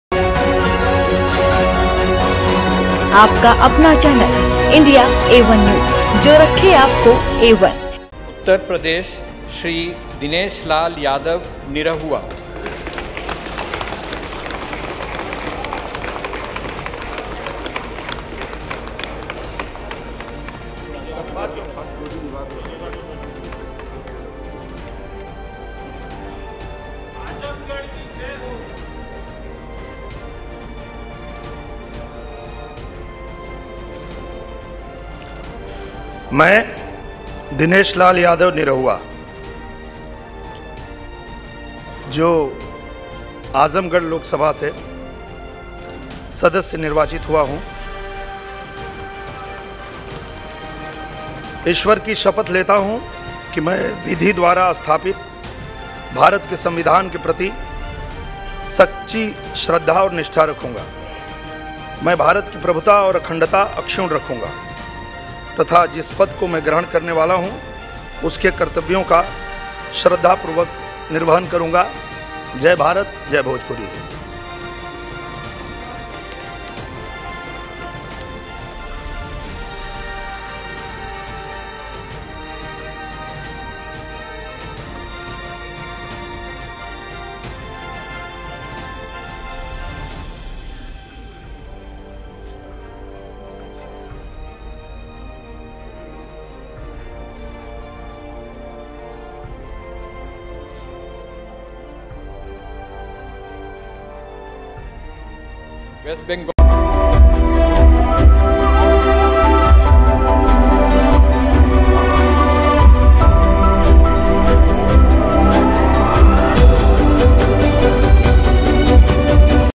Dinesh Lal Yadav Nirahua Oath : लोकसभा में आजमगढ़ के MP निरहुआ ने ली शपथ
उत्तर प्रदेश के लोकसभा उपचुनाव में आजमगढ़ सीट से निरहुआ ने जीत हासिल की। निरहुआ ने भारतीय जनता पार्टी से चुनाव लड़ा और जीत हासिल की। लोकसभा में निरहुआ ने शपथ ली और शपथ के बाद लोकसभा स्पीकर के पैर भी छए।